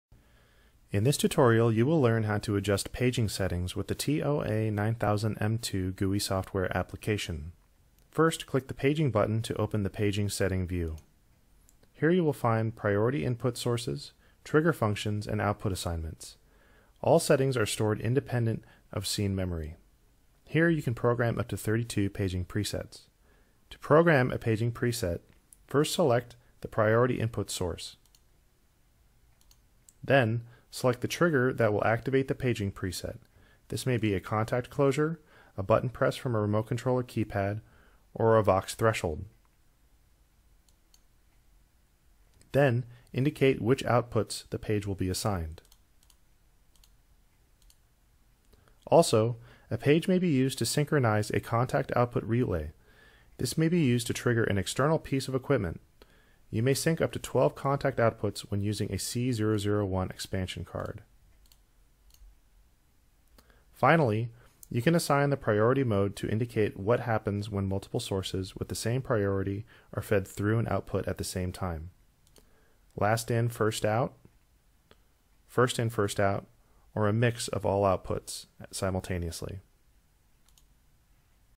TOA 9000M2 Software Part 11 - Paging Settings
In this tutorial you will learn how to adjust Paging settings  with the TOA 9000m2 GUI Software application.

Click the PAGING Button to open the PAGING Setting View. Here you will find priority input sources, trigger functions and output assignments. All settings are stored independent of scene memory.

Here You can program up to 32 Paging presets.

To program a Paging Preset, first select the priority input Source.

Then, Select the Trigger that will activate the paging preset--this may be a contact closure, a button press from a remote controller keypad, or Vox threshold.

Then, indicate which outputs the page will be assigned.

Also, a page can be used to synchronize a contact output relay, this may be useful to trigger an external piece of equipment. You may sync up to 12 contact outputs when using the C-001 Expansion Card.

Finally, you can assign the priority mode to indicate what happens when multiple sources with the same priority are fed through an output at the same time: Last in First out, First in First out, or all sources mixed at the same time.